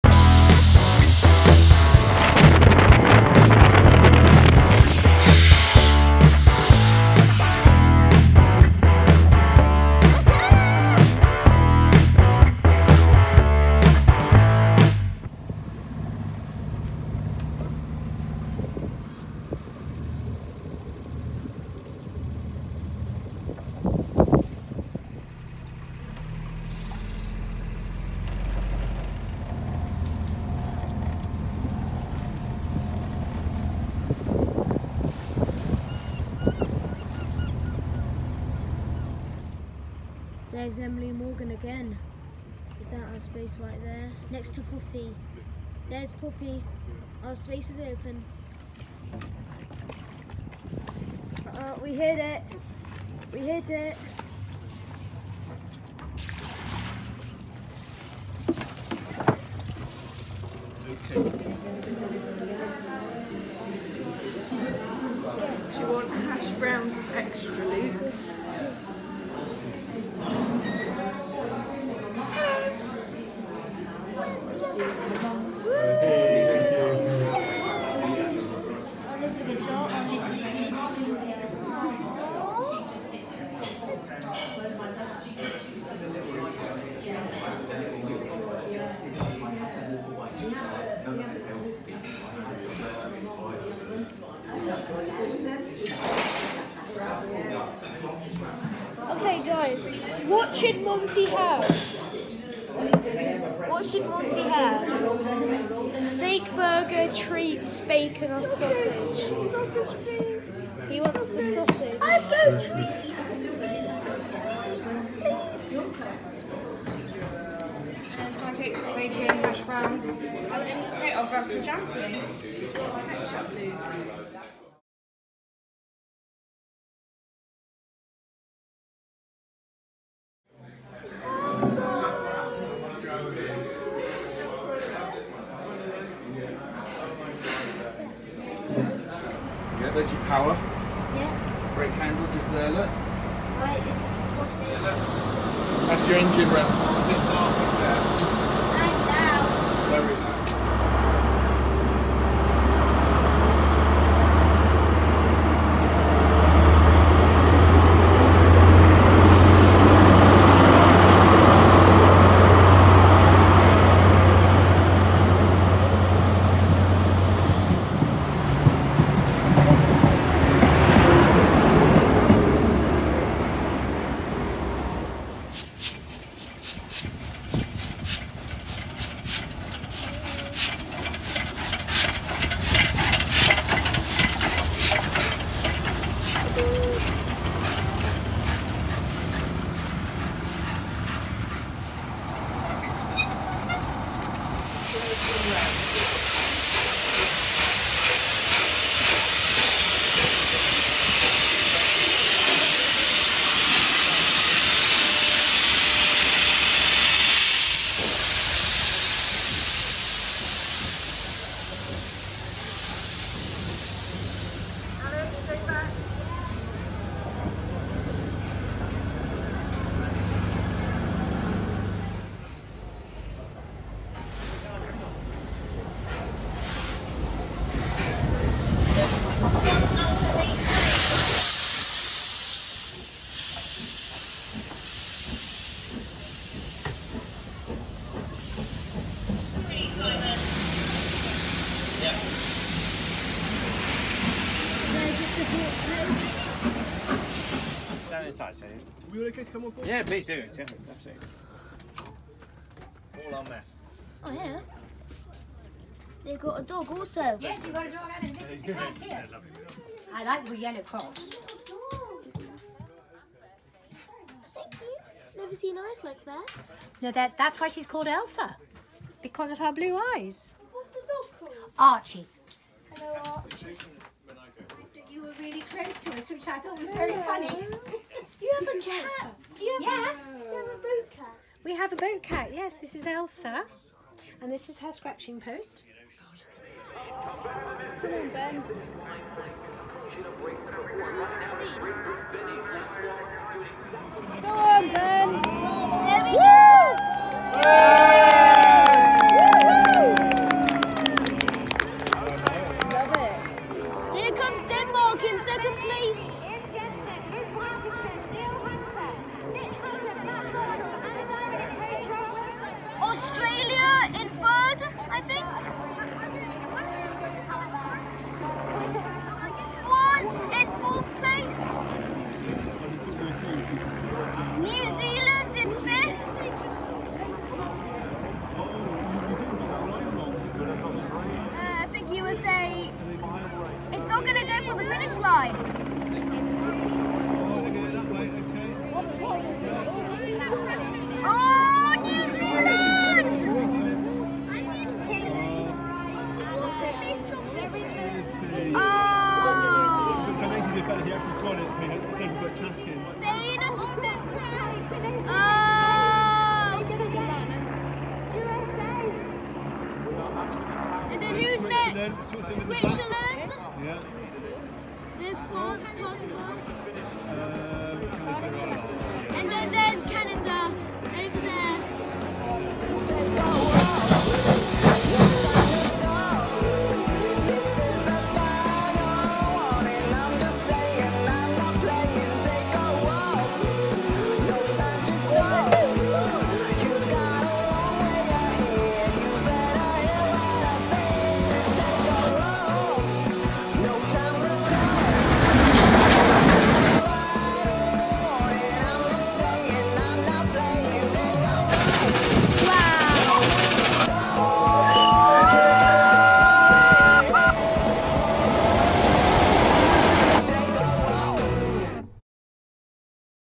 The 1st Adventure of Moonshadow Part 3 | LMS steam train & boat race
We say goodbye to Athena and head to QAB Marina, we also see Coronation class Duchess of Sutherland and a SAIL GP boat race.